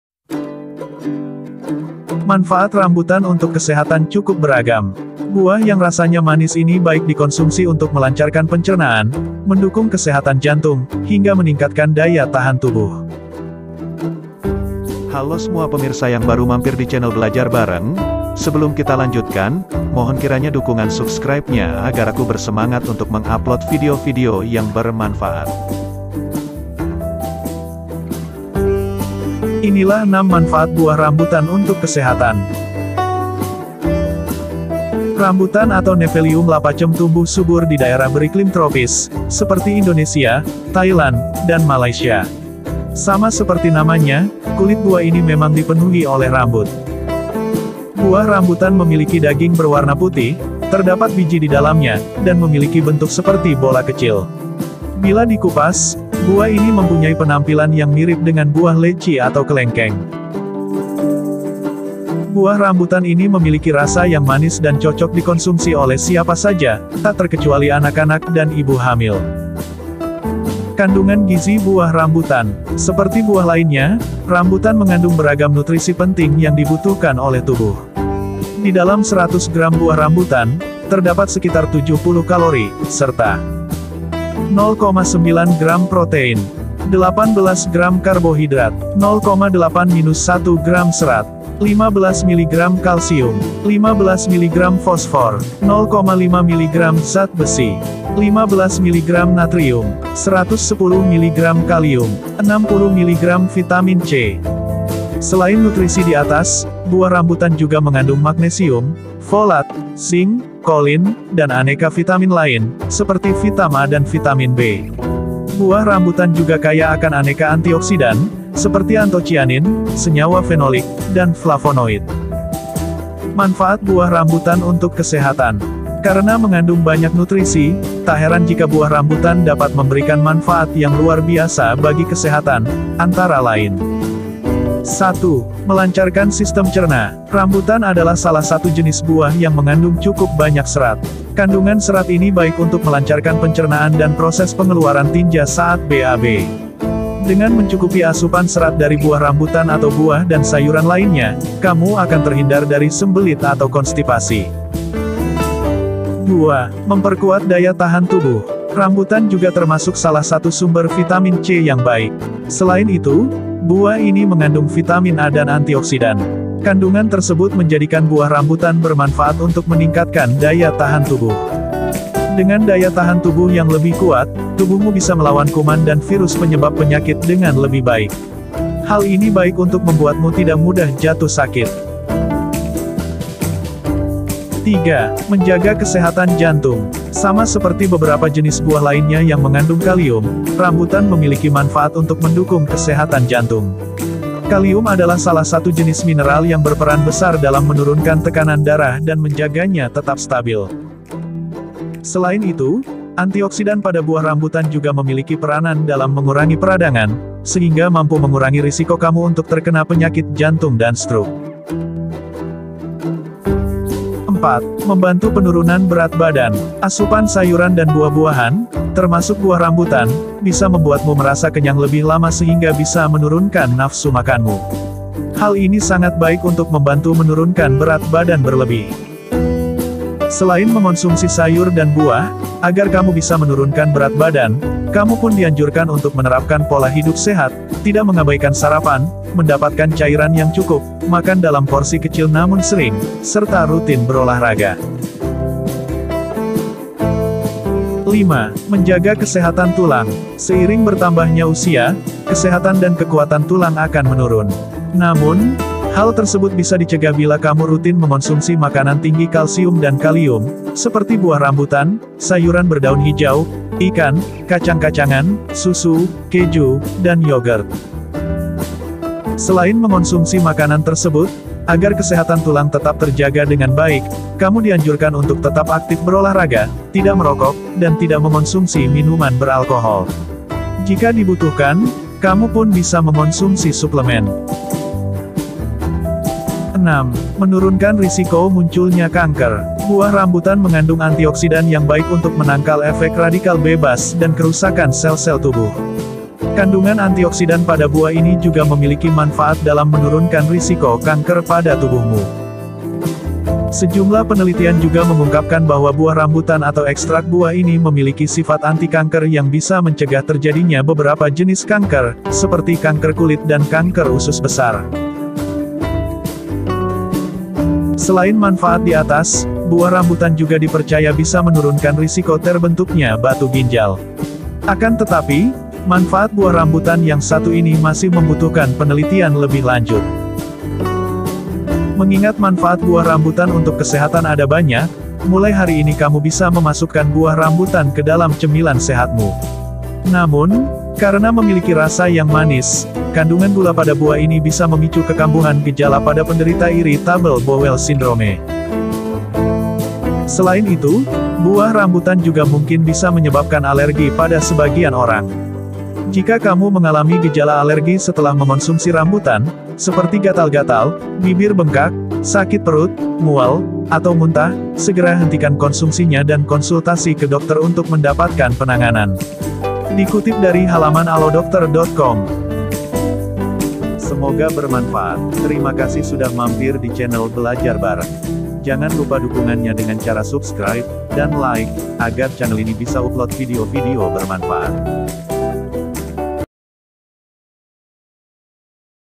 6 Manfaat Buah Rambutan untuk Kesehatan ‼️
6 Manfaat Buah Rambutan untuk Kesehatan

belajar bareng

Upload video :
5 januari 2022

Manfaat rambutan untuk kesehatan cukup beragam. Buah yang rasanya manis ini baik dikonsumsi untuk melancarkan pencernaan, mendukung kesehatan jantung, hingga meningkatkan daya tahan tubuh.
Rambutan atau Nephelium lappaceum tumbuh subur di daerah beriklim tropis, seperti Indonesia, Thailand, dan Malaysia. Sama seperti namanya, kulit buah ini memang dipenuhi oleh rambut.
Buah rambutan memiliki daging berwarna putih, terdapat biji di dalamnya, dan memiliki bentuk seperti bola kecil. Bila dikupas, buah ini mempunyai penampilan yang mirip dengan buah leci atau kelengkeng.
Buah rambutan ini memiliki rasa yang manis dan cocok dikonsumsi oleh siapa saja, tak terkecuali anak-anak dan ibu hamil.
Kandungan Gizi Buah Rambutan
Seperti buah lainnya, rambutan mengandung beragam nutrisi penting yang dibutuhkan oleh tubuh. Di dalam 100 gram buah rambutan, terdapat sekitar 70 kalori serta:

0,9 gram protein
18 gram karbohidrat
0,8–1 gram serat
15 miligram kalsium
15 miligram fosfor
0,5 miligram zat besi
15 miligram natrium
110 miligram kalium
60 miligram vitamin C